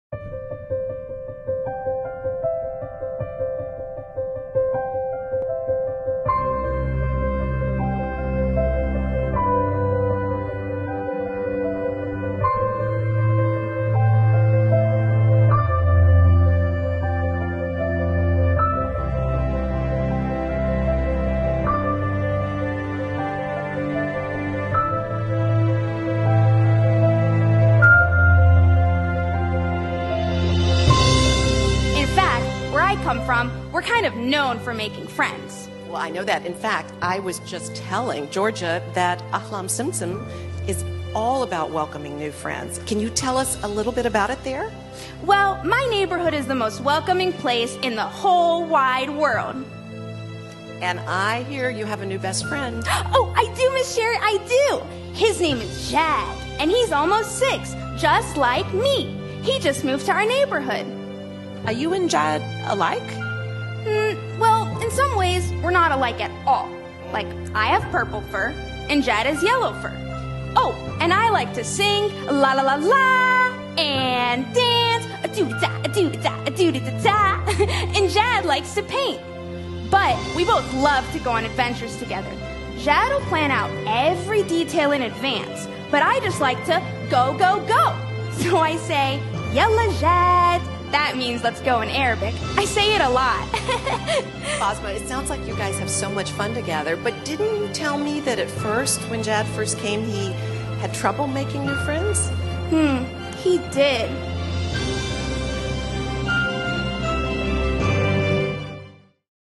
Meet Basma in Sesame Street Ahlan Simsim Series to Support Child Refugees
Sesame Workshop, the non-profit organization behind the Sesame Street show, has teamed up with the International Rescue Committee (IRC) to produce an entire season of episodes with the aim of bringing a smile to refugee children's faces, help them learn the alphabet and develop numerical skills – but also better process loneliness, fear and despair.

For over 50 years, the characters of Sesame Street, from the Cookie Monster to Big Bird, have helped children from diverse backgrounds navigate the challenges of life as a small person in a big world.

At the SDG MEDIA ZONE, during a panel session , Sherrie Westin,  the director of Global Impact and Philanthropy for Sesame Workshop in the United States, said they endeavored to ensure the new Sesame Street show features relatable situations and characters from the Middle East to truly connect with refugee kids. She then announced the launch of Ahlan Simsim (“Welcome Sesame” in Arabic) where we got introduced to the BASMA character.